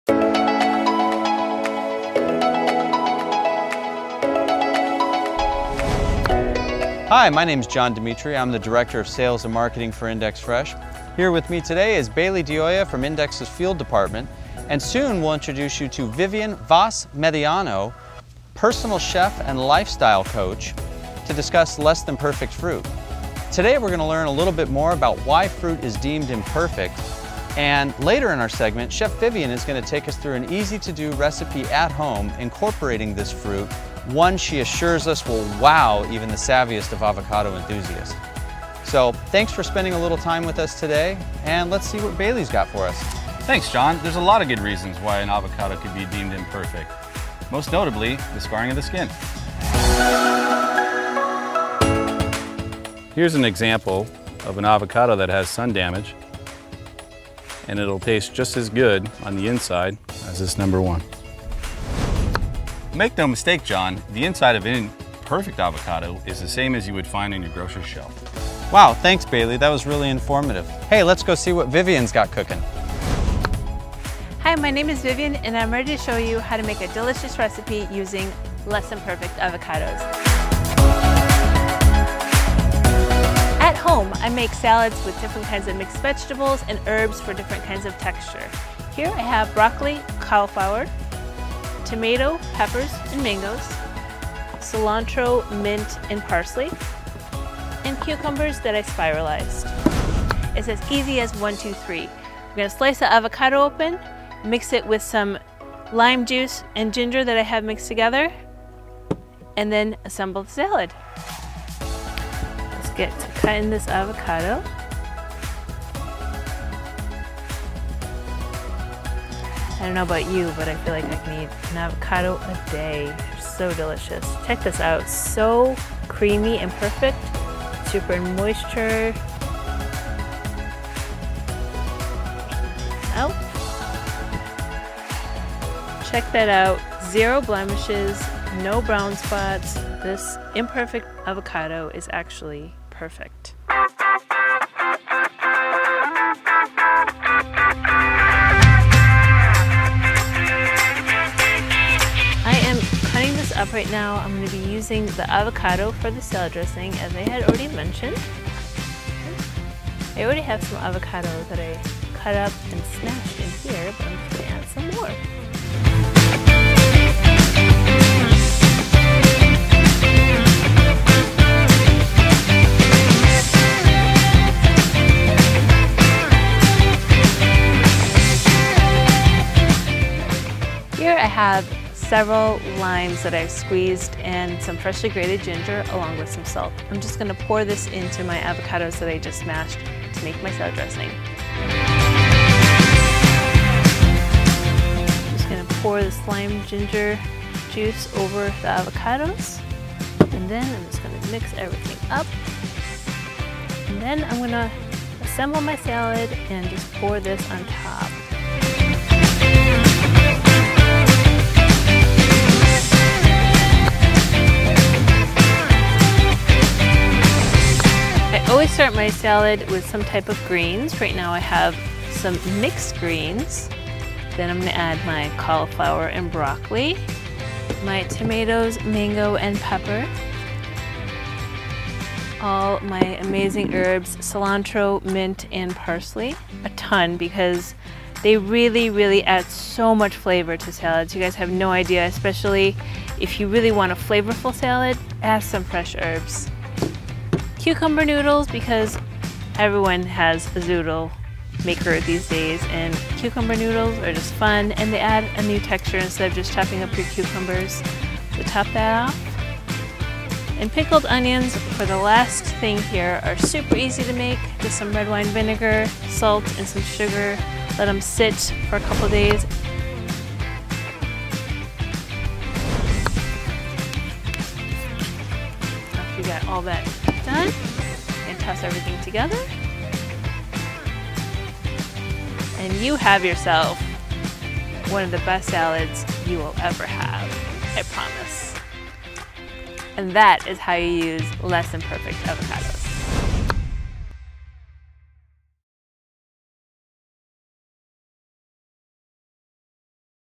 So Good Avocados, #2's
Learn how our #2 brand avocados are perfect on the inside!